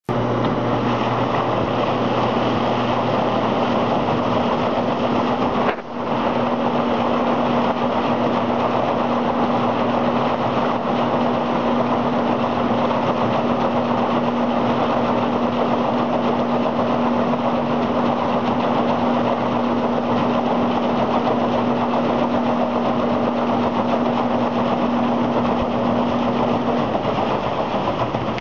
Water wheel
This water pump is driven by a 1 horse motor That will turn a water wheel and provide more than enough water to turn a generator.